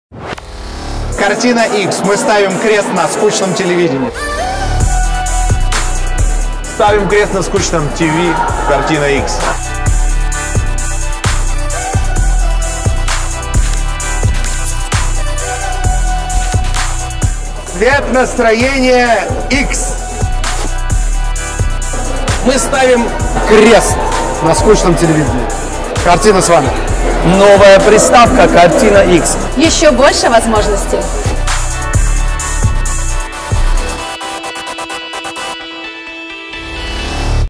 Крид, Лазарев, Киркоров, Лорак, EMIN, Панайотов выбирают KartinaX | Презентация на премии ЖАРА 2019
Kartina.TV ставит КРЕСТ на скучном телевидении.
Презентация приставки нового поколения KartinaX на ЖАРА MUSIC AWARDS 2019
Егор Крид, Филипп Киркоров, Ани Лорак, Сергей Лазарев, EMIN, Александр Панайотов, Brandon Stone, Artik, Артем Качер, Группа Пицца, Миша Марвин и огромное количество довольных клиентов уже оценили её функционал. Осталось дело за тобой!